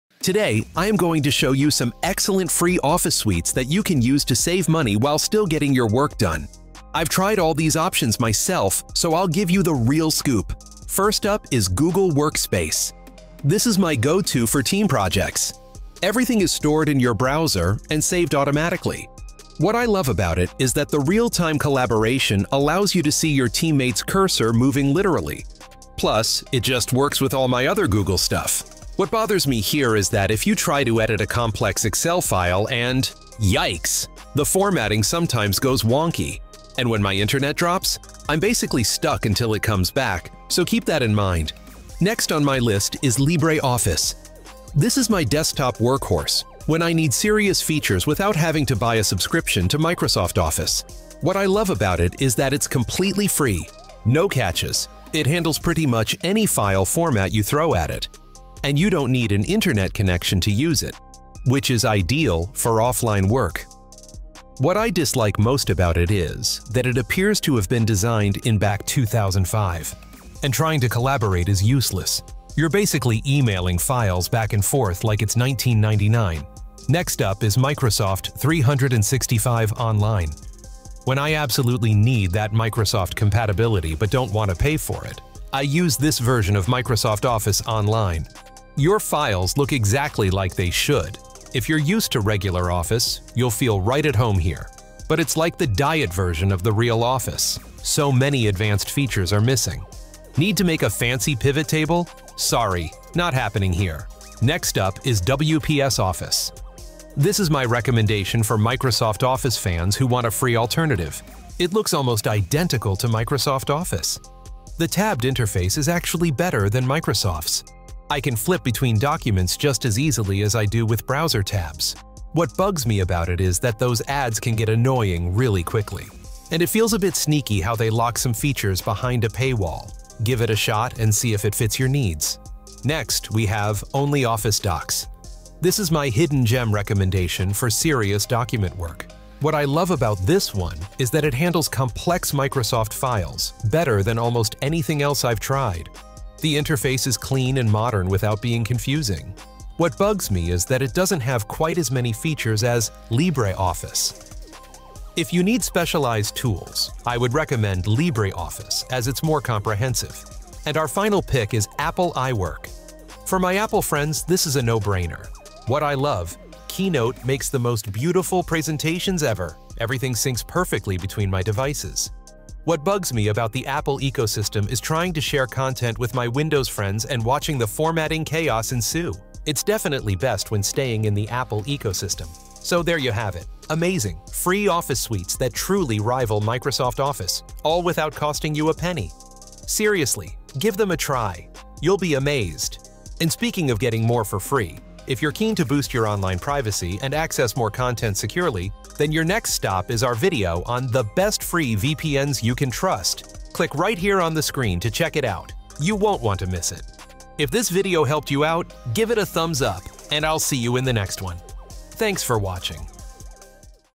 These Free Office Tools Will Replace Microsoft 365 (2026 Update)
👉 Download Best FREE Microsoft Office Alternative

This video reveals the Best Free Microsoft Office Alternatives that can rival—and even beat—Microsoft Office in 2026! Discover top alternatives like Google Workspace, LibreOffice, WPS Office, OnlyOffice Docs, Microsoft 365 Online, and Apple iWork. Whether you need powerful offline editing, seamless collaboration, or a familiar interface, there’s a perfect free office suite for you. No subscriptions, no hidden fees—just real productivity! After watching, you’ll know exactly which free office suite fits your workflow and how to get started. This video reviews several free office suites, highlighting their features and how they can help users save money. Discover top alternatives like Google Docs and LibreOffice to enhance your productivity. Explore the best free Microsoft Office alternatives to streamline your workflow.

Free Office Suites Better Than Microsoft Office? Top Picks! | Honest Review & Guide

Or do you have a favorite that we missed? Let us know!

today!